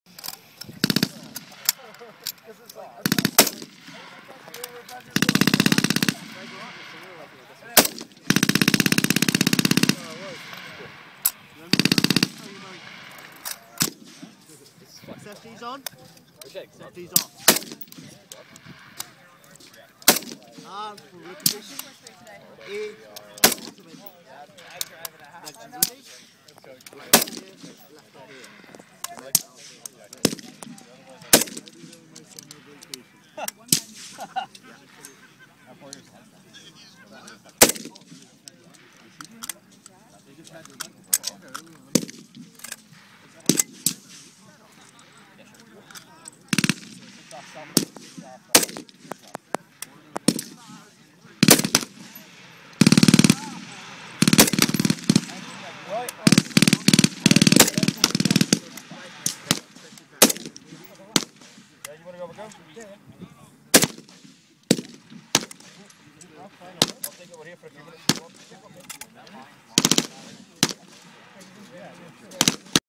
Myself Firing a British SA80 service rifle
Firing a SA80 Service Rifle, main rifle with the british army via YouTube Capture